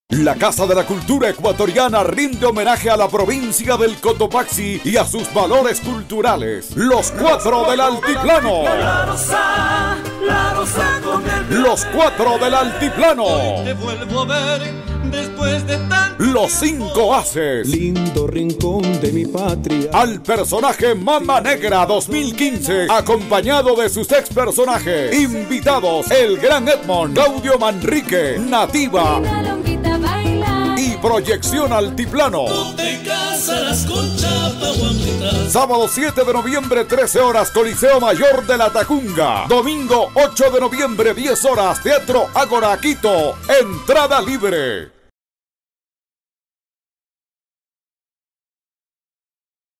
DOMINGOS DE CASA ABIERTA NOVIEMBRE
Les invitamos a Domingos de Casa Abierta este 7 de noviembre en el Coliseo Mayor de Latacunga, 13:00 y el domingo 8 en el Ágora de la CCE, 10:h00